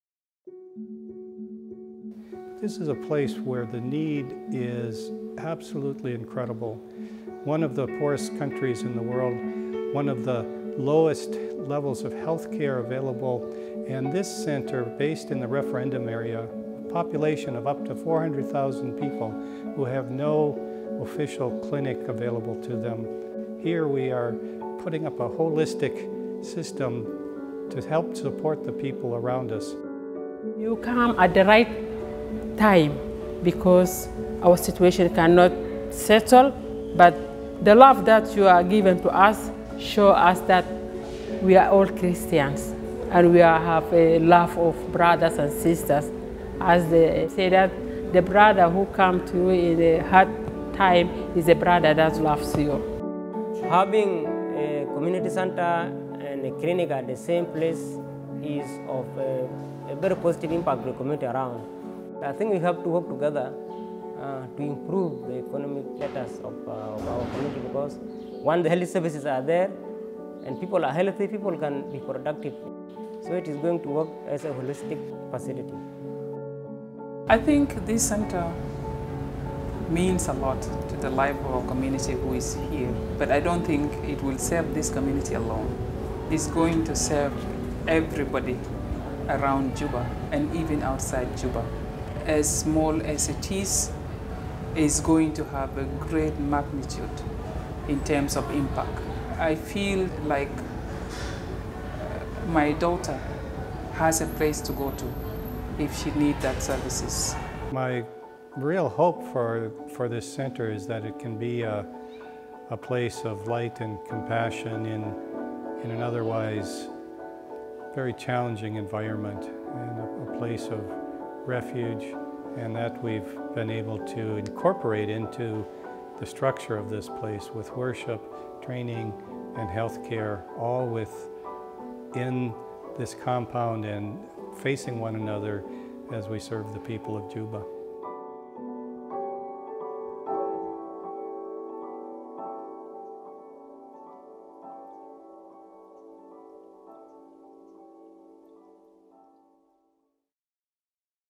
Hope Rising in Juba, South Sudan | ELCA
In 2015 an idea was born that brought together people from across South Sudan and the ELCA. After two years of prayer and partnership the Reconciliation Lutheran Church Community Center and Primary Health Care Clinic opened its doors in November of 2017 as thousands gathered to worship and celebrate.